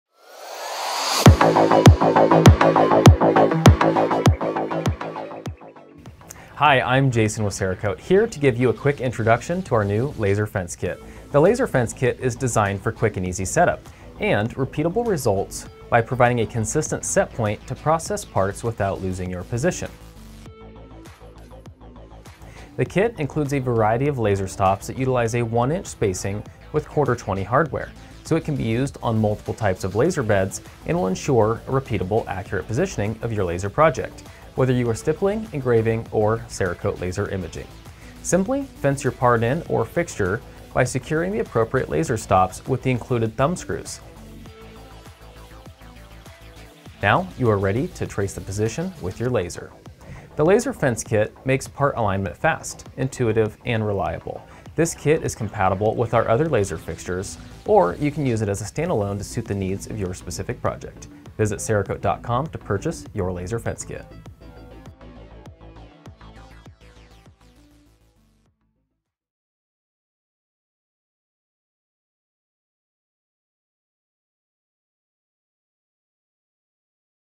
Product Spotlight: Laser Fence Kit | CERAKOTE®
We designed the Laser Fence Kit to allow quick and easy setup and to provide a repeatable set point to process parts without losing your position for your Laser Imaging projects. This kit utilizes 1” spacing and ¼-20 hardware and is compatible with multiple types of laser beds.

Easily attachable to the laser bed with the included thumb screws, this adjustable kit creates a structured layout for quick, consistent part placement, reducing setup time and improving workflow efficiency. Ideal for precise and repeatable laser stippling, engraving, or Cerakote laser imaging, the Laser Fence Kit enhances productivity by making part alignment fast, intuitive, and reliable.

All laser stops are 1" wide and 5/16" tall. Holes are spaced either 1" or 2" apart, with slots included for additional screws or angled mounting.

Kit Contains:
• 8 - Straight Laser Stop (2-2", 4-3", 2-5")
• 4 - Corner Laser Stop
• 2 - Tee Laser Stop
• 4 - Sliding Clamp/Stop
• 1 - V-Block
• Hex key, thumb and button head screws
• Storage Box